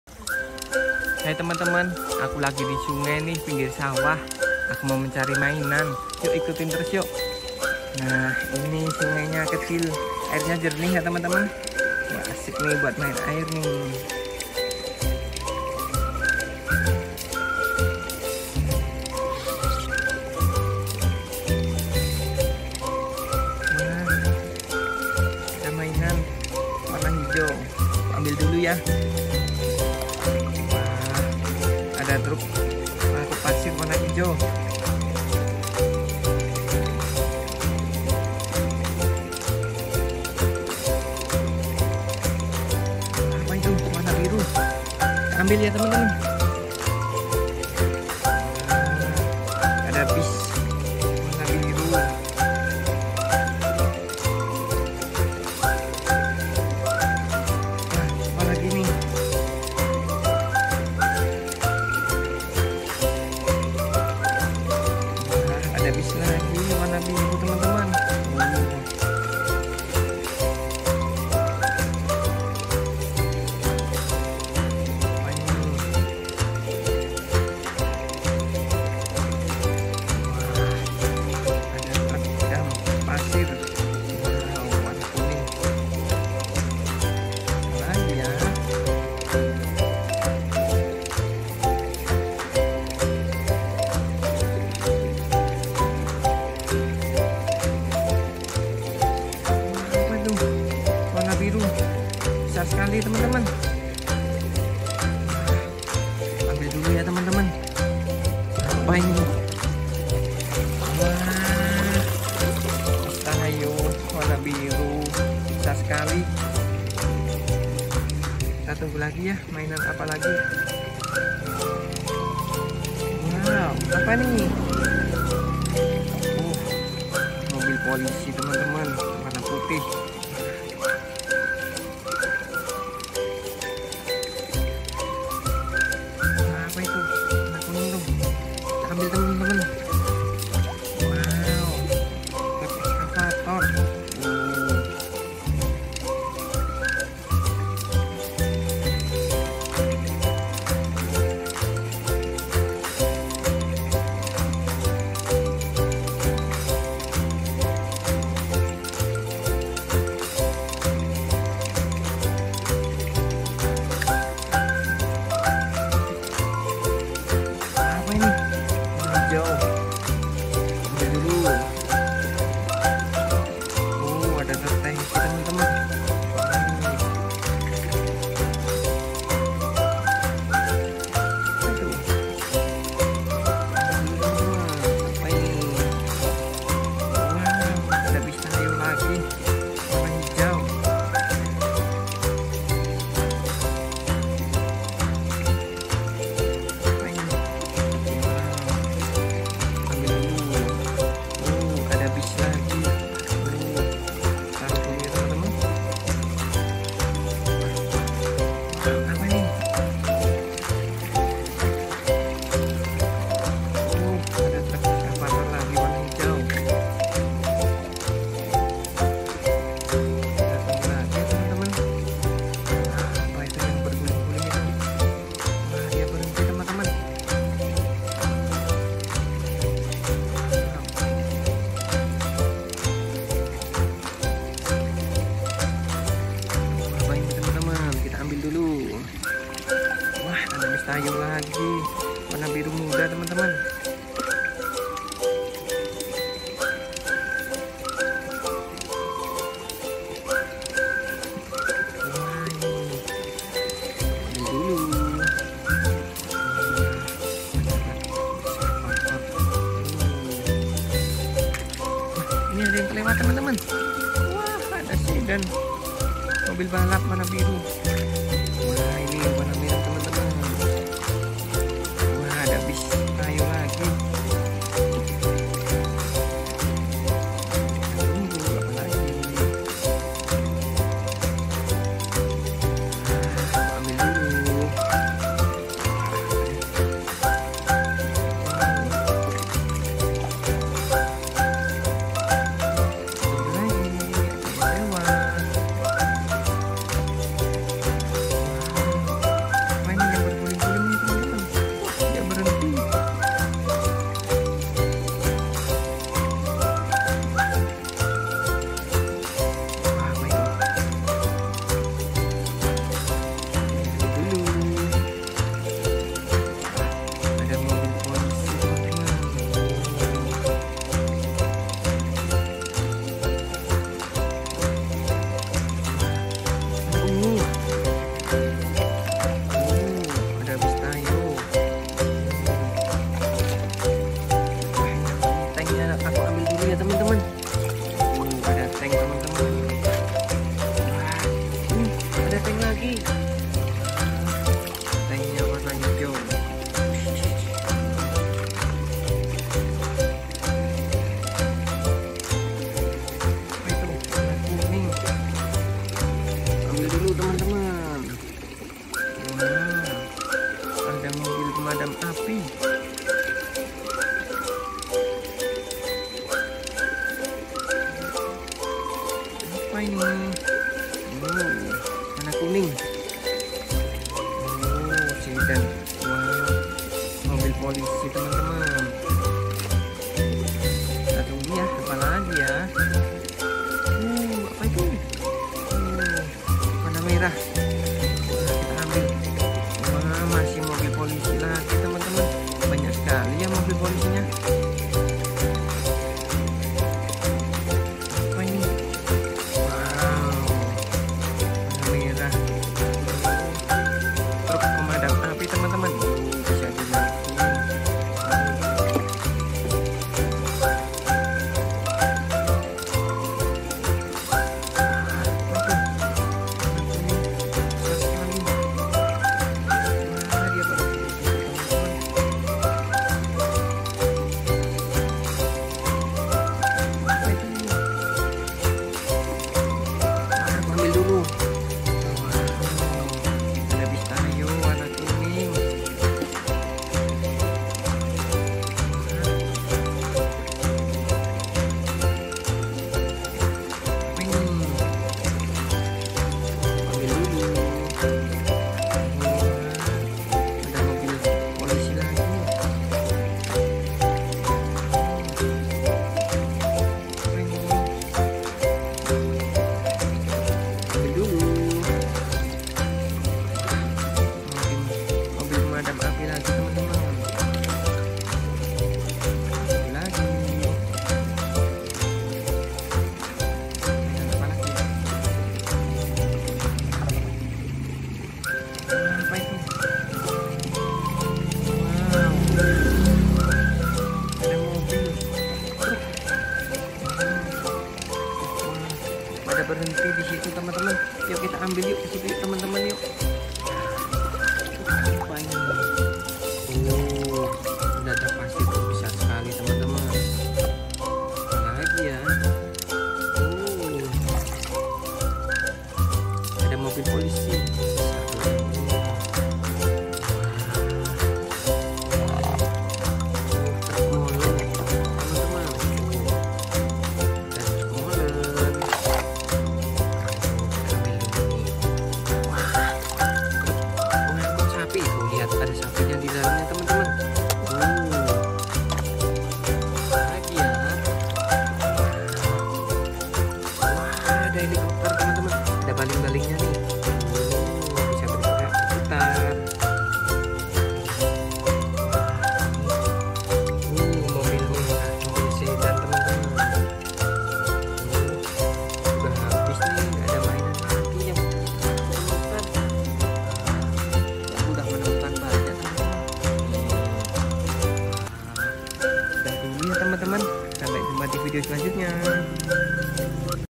Mencari Mobil, Tank, Helikopter Mainan di Sungai
Video Kidomi Indonesia kali ini menampilkan mencari mainan mobil polisi, bus, truk, tank, helikopter, dan masih banyak mobil mainan lainnya di sungai.

Selamat menonton video dari Kidomi Indonesia , terimakasih :)